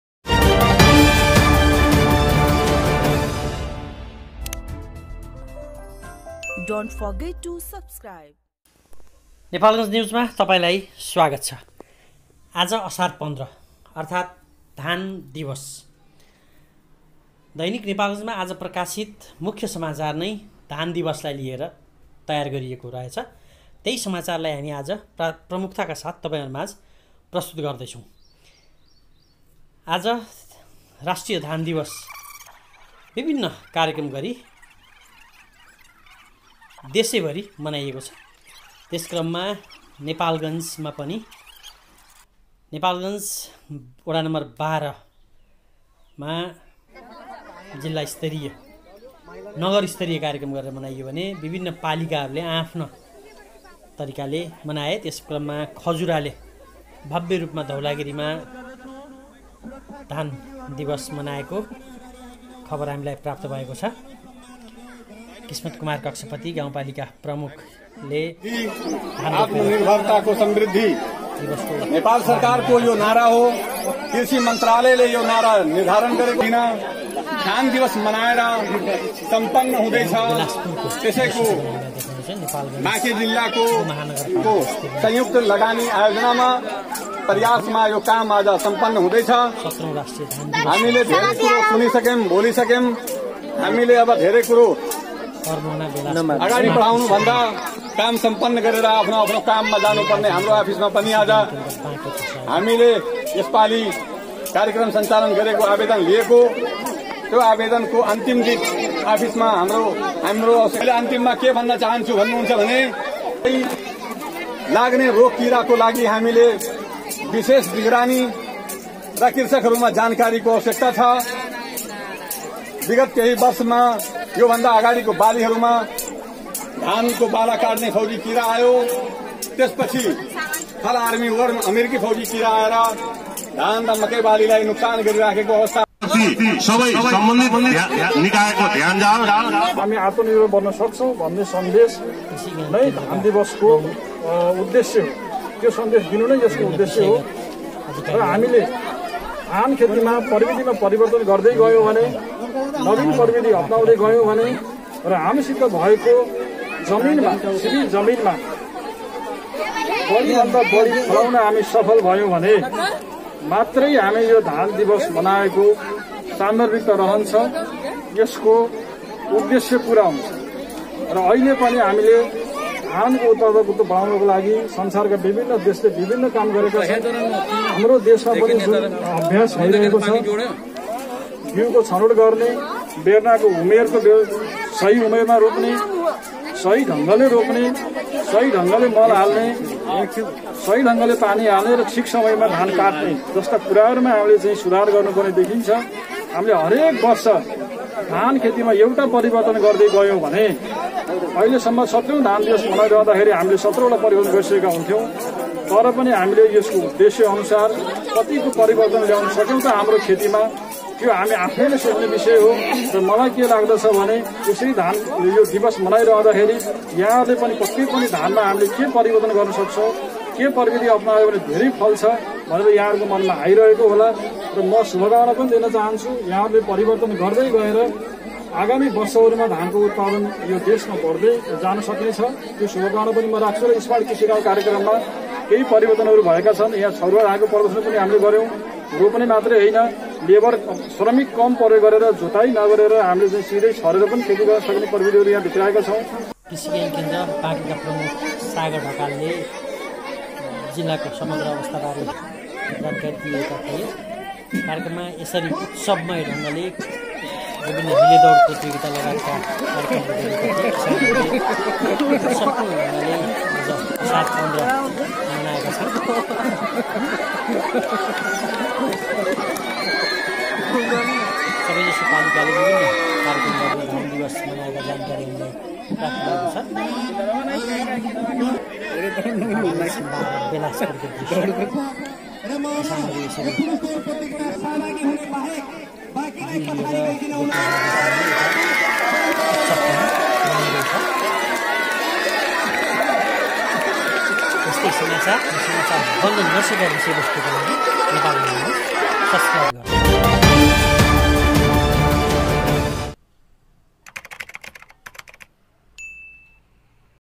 नेपालगन्ज: धुमधाम धान दिवस
असार १५ । आज सोमबार विभिन्न कार्यक्रम गरि धान दिवस मनाइएको छ ।